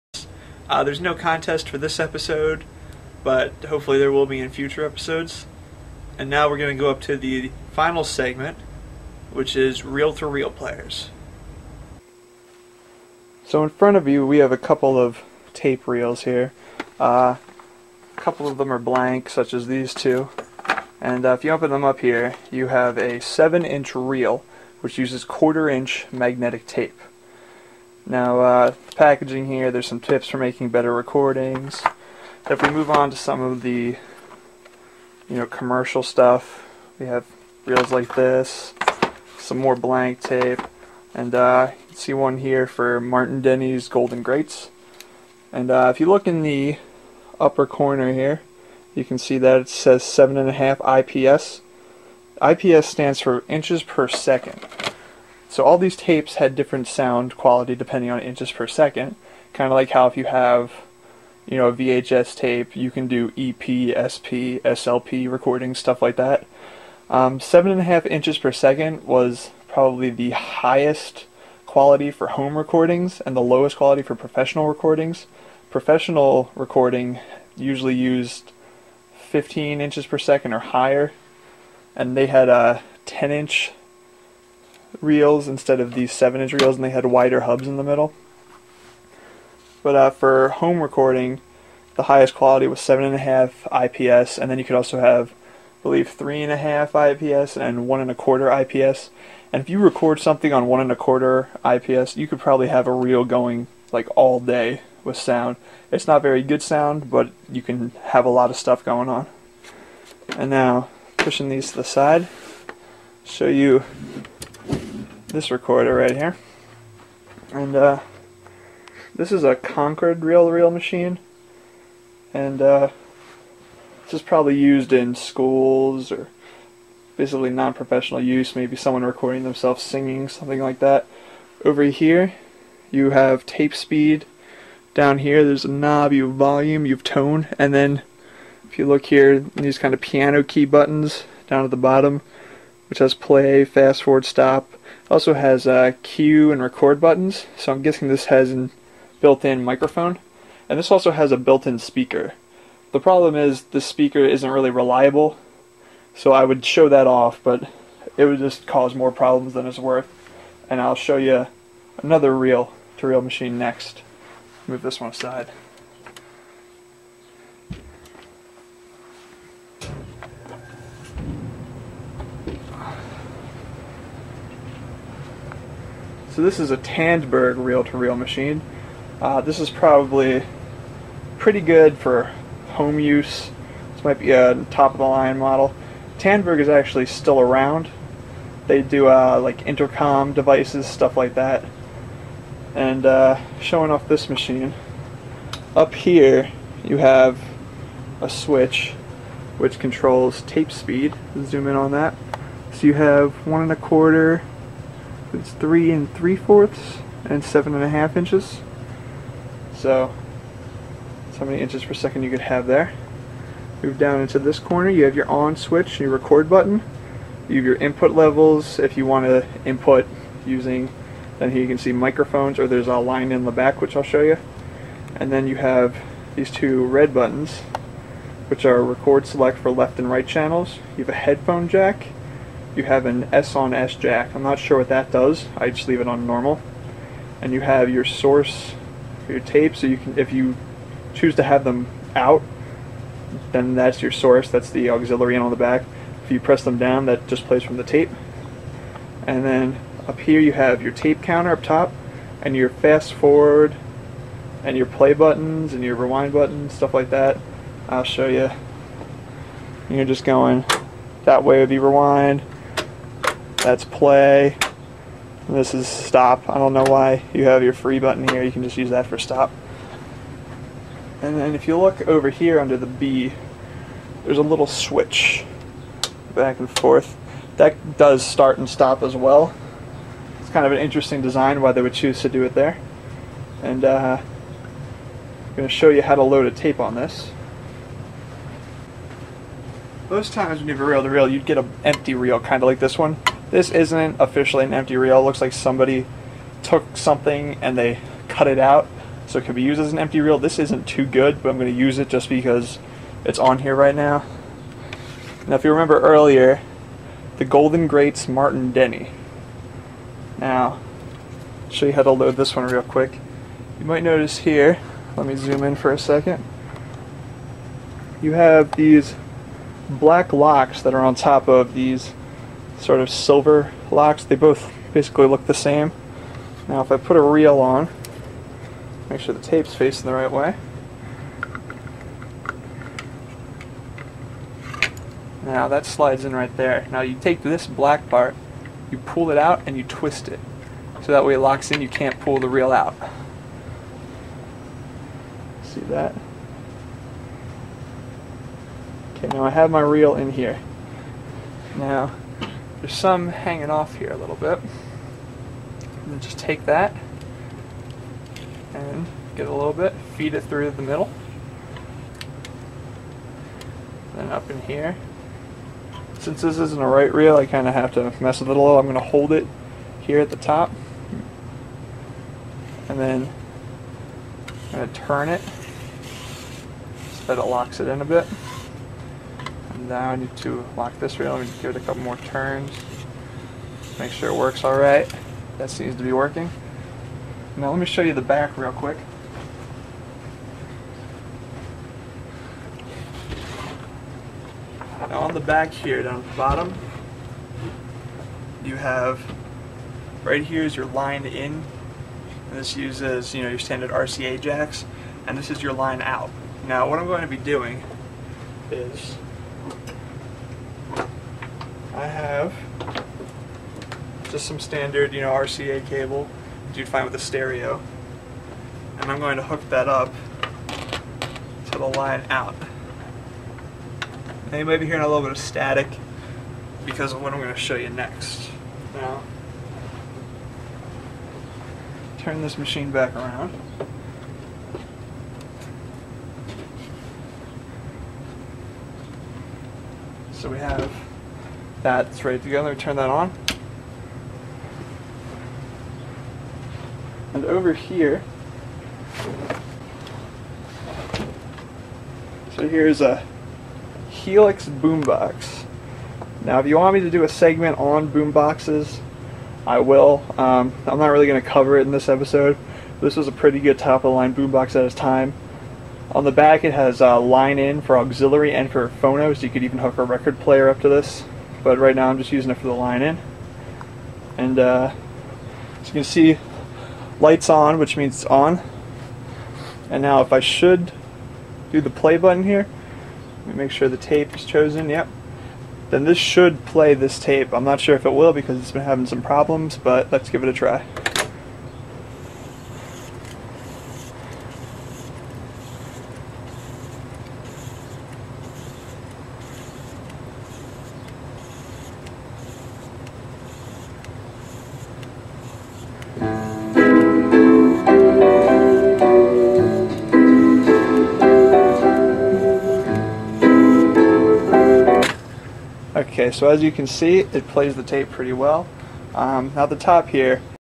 Obsoleet s01e03 -- Part 3/4
Segments:
Betamax Basics
Vocoding with the Stylophone
Reel-to-Reel Players

"Betamax Basics" used a Sanyo Betacord VCR 4400, and a broken Sony Betamax SL-HF550. The blank tapes were Kodak and Sony brands with the number L-750. This segment focused on using the Betamax player while providing a brief history.

"Vocoding with the Stylophone" used a Stylophone available from Thinkgeek, The Stylophone was hooked through the computer using the headphone jack on the stylophone and the line-in jack on my computer's sound card. Programs featured in the segment were Audacity . The segment talked briefly about the Stylophone's use as an instrument and then demonstrated the ability to use it as a post-recording vocoder tool in the style of bands such as Daft Punk and Electric Light Orchestra.

"Reel-to-Reel Players" used a Concord Tape Recorder, a Tandberg 3300X, and an Akai GX-2300. This segment also made use of a Helix HX-4635 Boombox which is also known in other markets as a Conion C-100F and a Claritone 7980. An iPod Video was used briefly to show the recording capabilities. The segment mainly focused on showcasing the players as well as showing record and playback abilities.